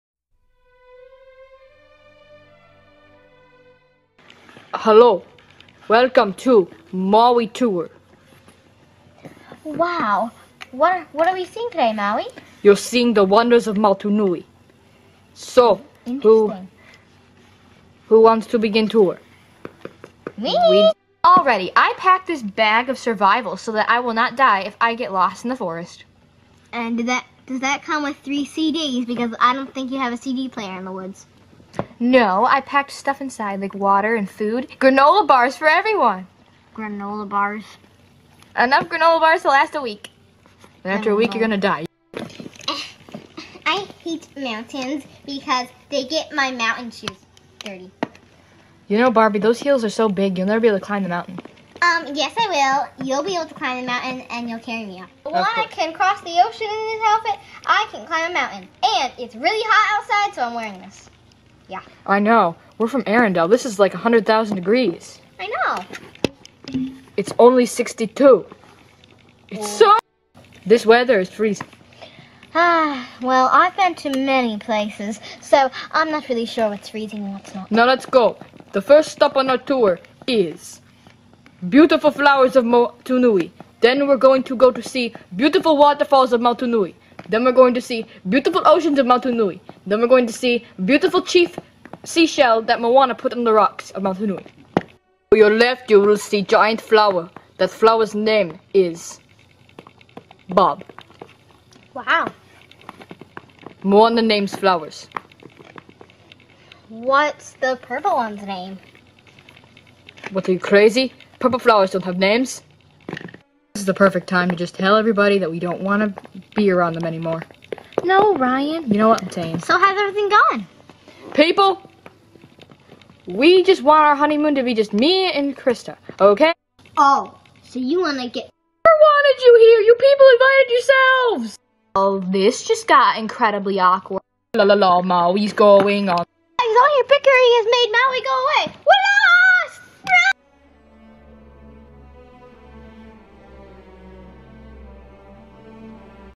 Frozen and Moana Krista and Ryan Honeymoon The Tour
In this video Maui is giving them a tour of Moana's island.